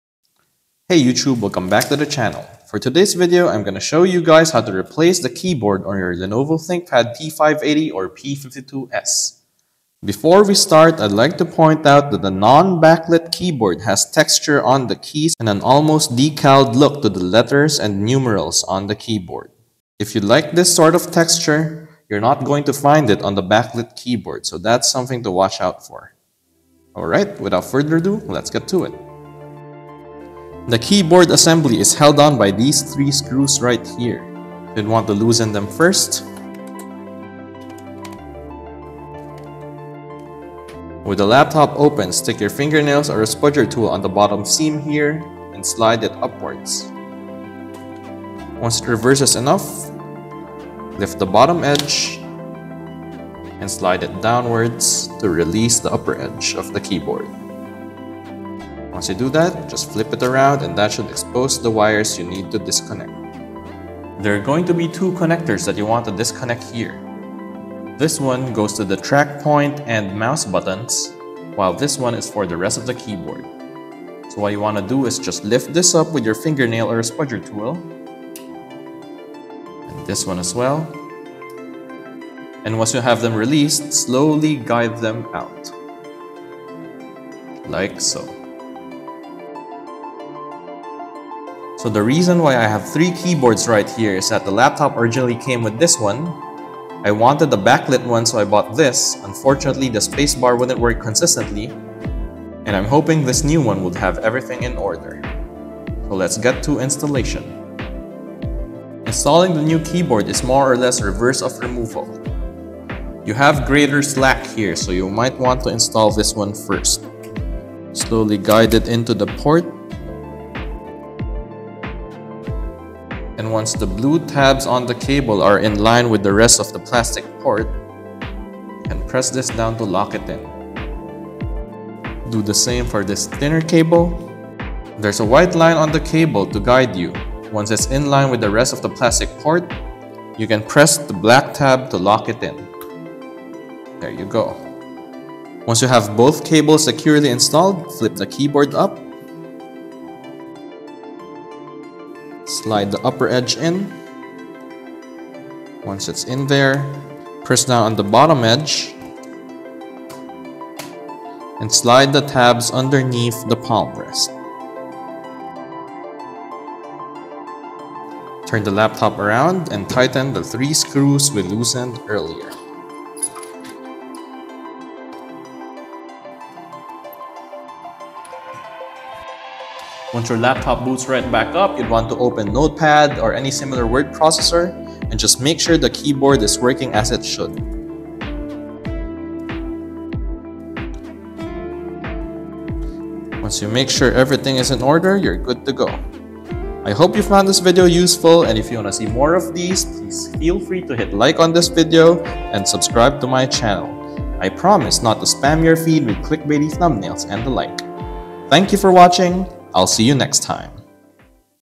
DIY Thinkpad T580 P52s Keyboard Replacement Guide | Lenovo
A DIY video guide on replacing the Lenovo Thinkpad T580 or P52s laptop's keyboard. Beginner-friendly with its step by step format.

00:00 - Intro
00:29 - Keyboard replacement
02:59 - Validation

Please feel free to drop a word in the comments and let me know how I can improve my future videos.